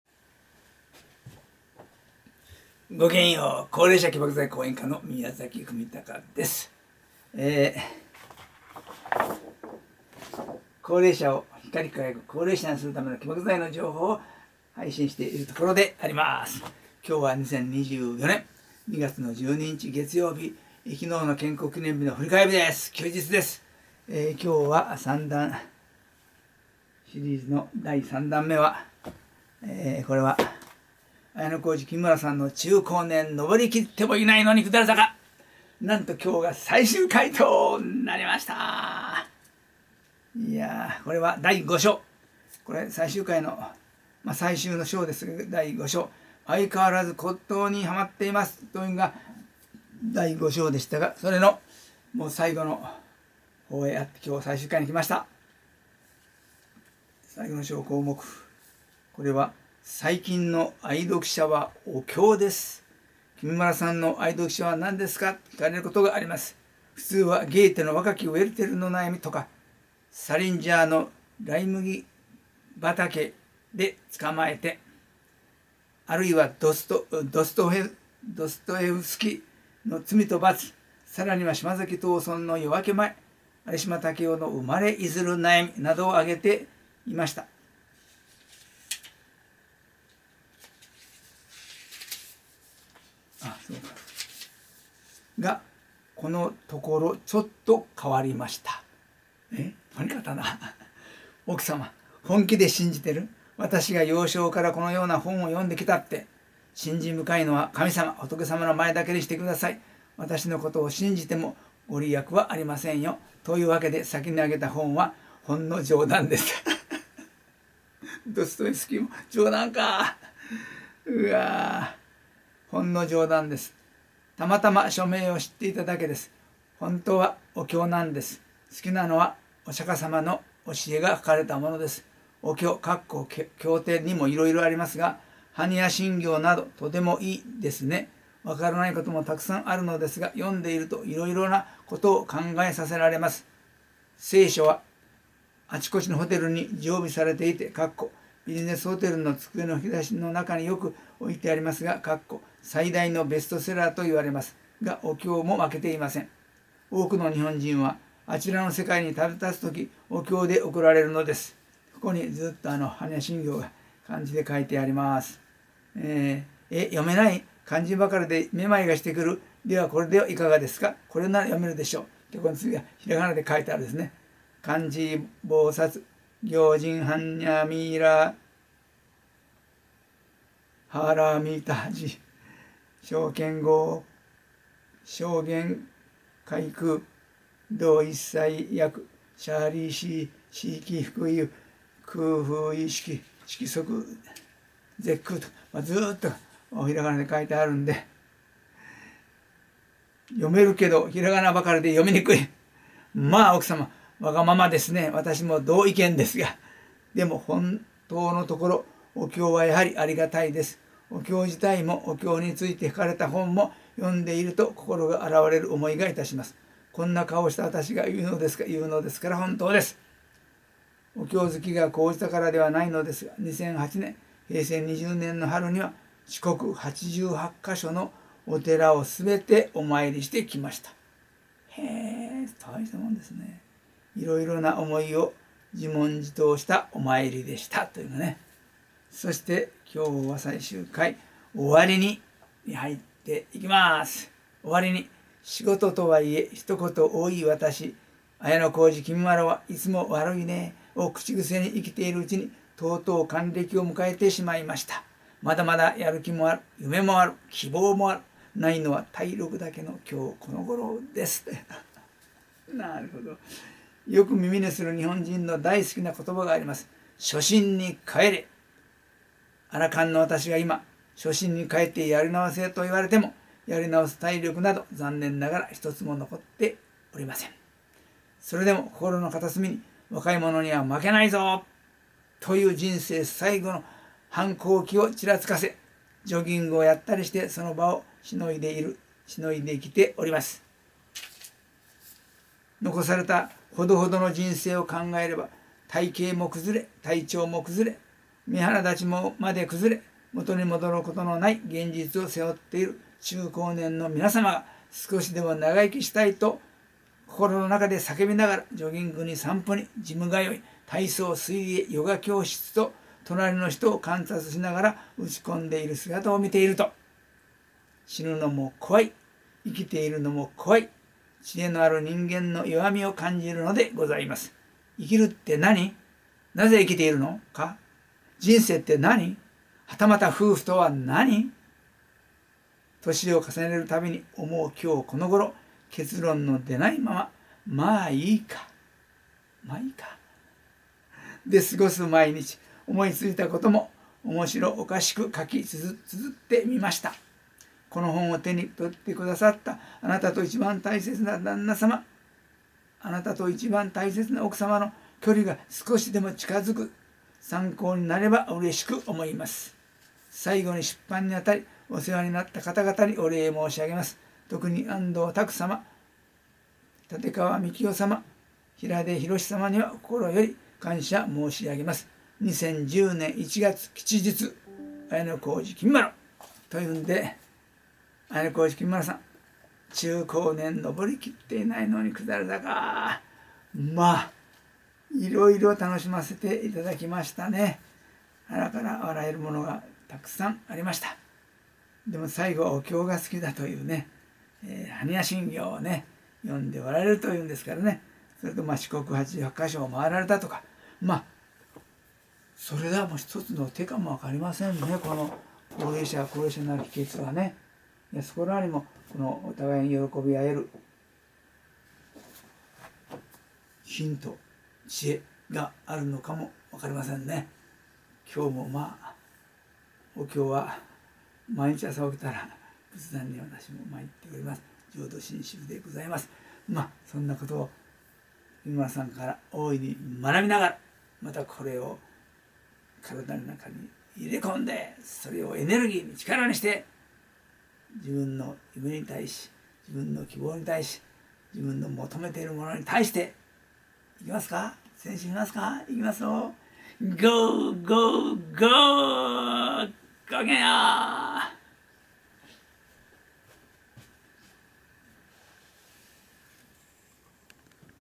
綾小路きみまろ 著 「中高年のぼりきっても いないのに下り坂」第33弾
若いころ「七転び八起き」の人生が、
いまでは「七転びやっと起き」に・・・・
第5章　相変わらず骨董にはまっています
　　　・最近の愛読書は「お経」です
　　　・おわり
2024年02月12日(月)

◆「光齢者志民会議」会長   ※光齢者「喜爆剤」講演家
◆「感動輝爆剤研究所」所長

ご支援よろしくお願いしま★